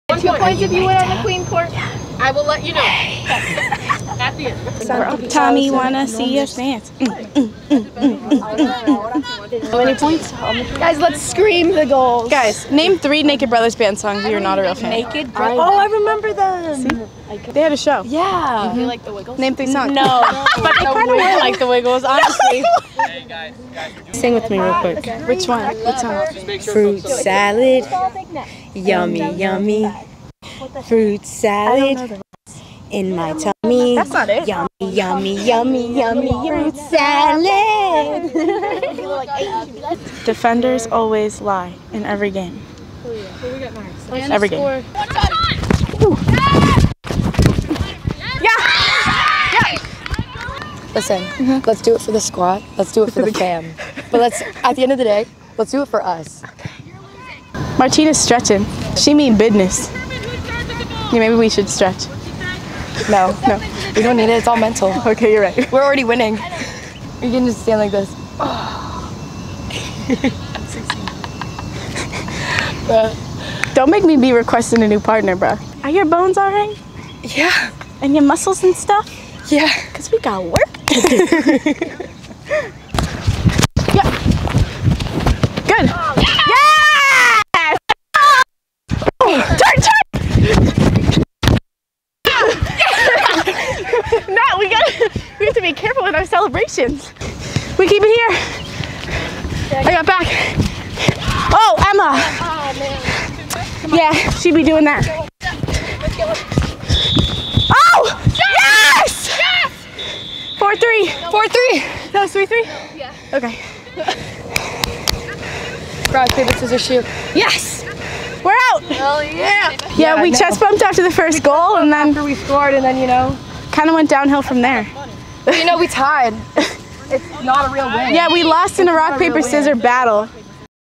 Mic'd Up with AU Women's Soccer - Tate Jordan
Come for Tate Jordan being Mic'd Up. Stay for Alysa Vazquez singing The Wiggles karaoke.